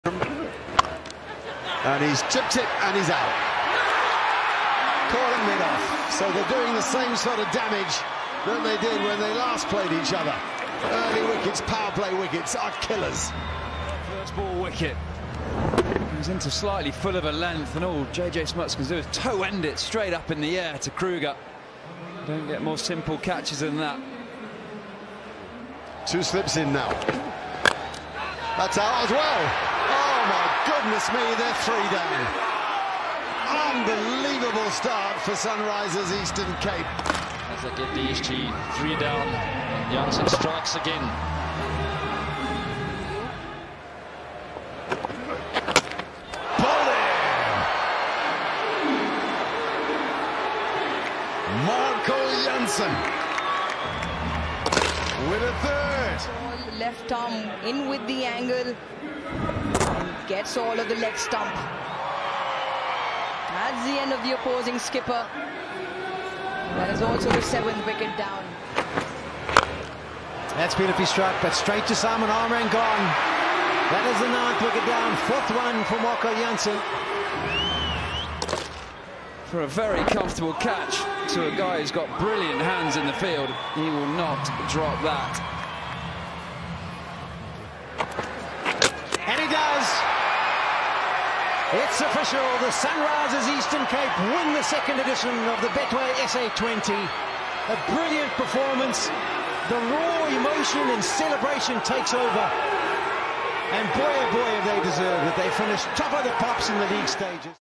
Incredible Marco Jansen | 5-30 in The Final | Sunrisers Eastern Cape v Durban's Super Giants
Sunrisers Eastern Cape's Marco Jansen claimed the third 'Michelle' of Betway SA20 Season 2, taking 5-30 in the Final against Durban's Super Giants.